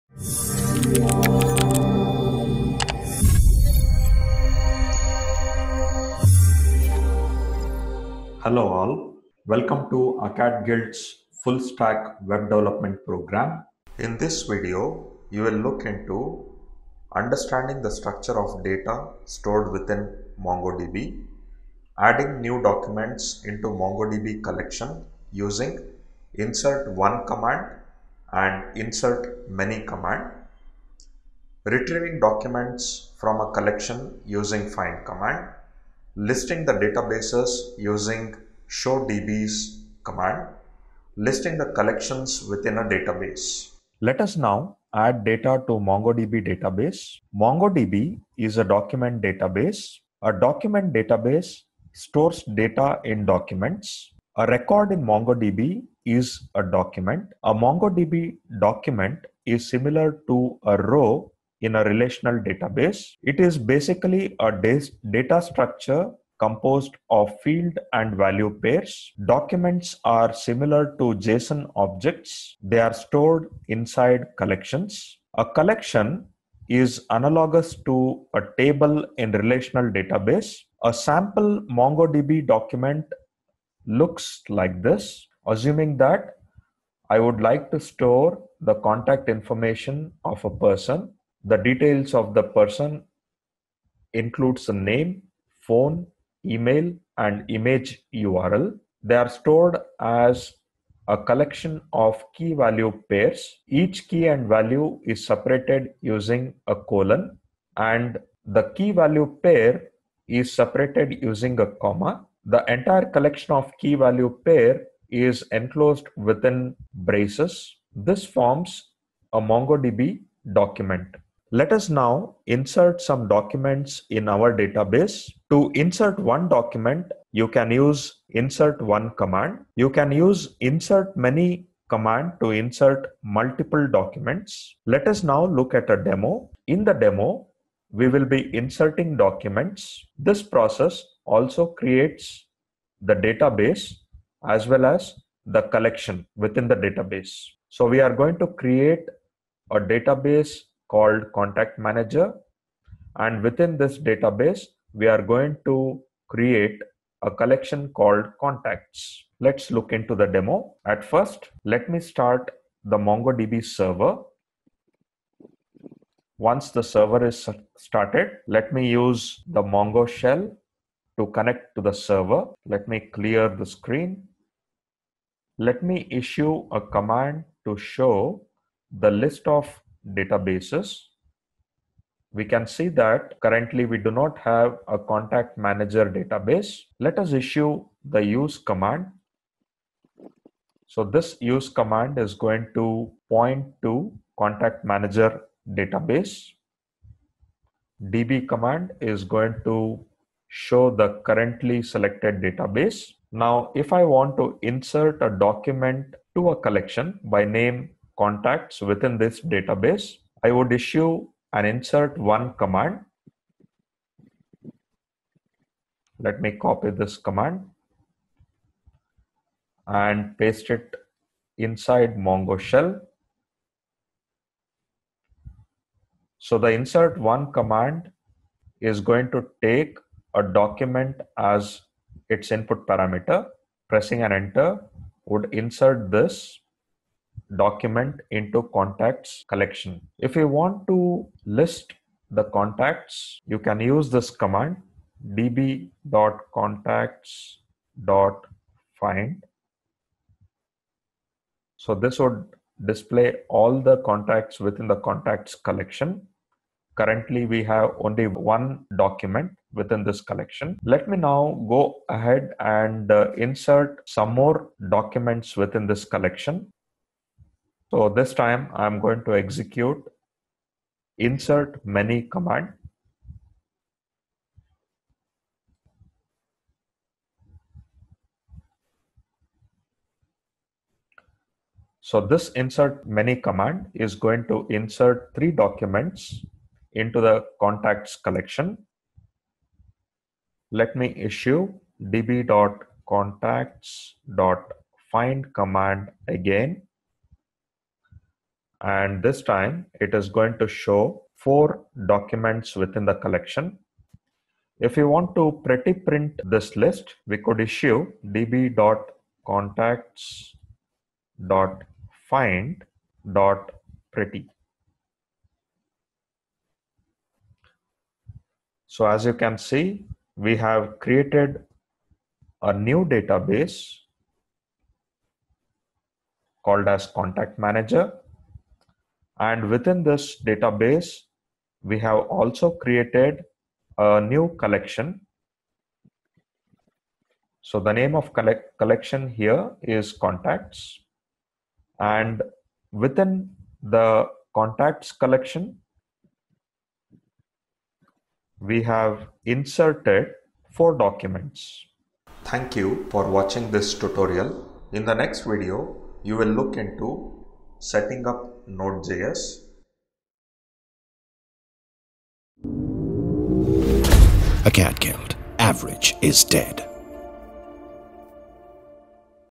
Full Stack Developer Tutorial #3 | How to Insert Data to MongoDB | Full Stack Web Developer Tutorial
Hello and welcome back to Full Stack Web Development tutorial series powered by Acadgild. In the previous sessions, we have discussed and learned the introduction part of full stack web development tutorial talking about the Project Overview and the Feature Listing and in the 2nd part, how to set up MongoDB. In this video, you will be able to learn, how to insert data into MongoDB.
Before that, if you have missed the previous sessions, please check out in the following links:

In this video You will look into:
•  Understanding the structure of data stored within MongoDB
•  Adding new documents into MongoDB collection using insert ‘one’ command and insert ‘many’ command
• Retrieving documents from our collections using the ‘find’ command
• Listing the databases using ‘showdbs’ command
• Listing the collections within a database

How to Add Data into MongoDB?
• Create a database called ContactManager
• Create a collection named Contacts.
• Add some documents to our Contacts collection